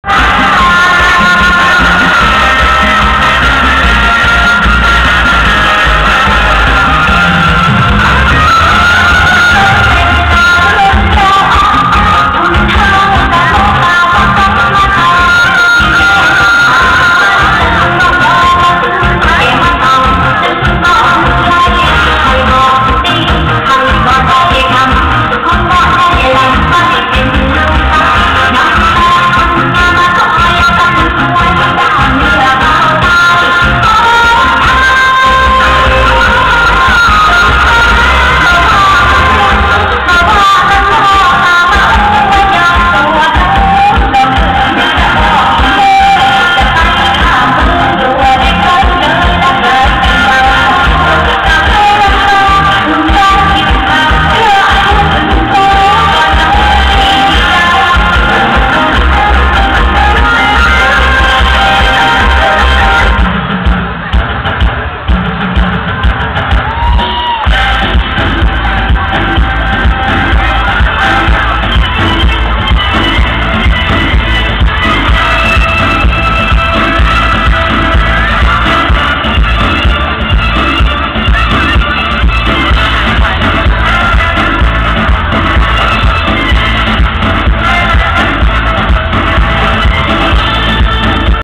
2nd Movie kids on State dancing at their Thai School
2nd Movie kids on State dancing at their Thai School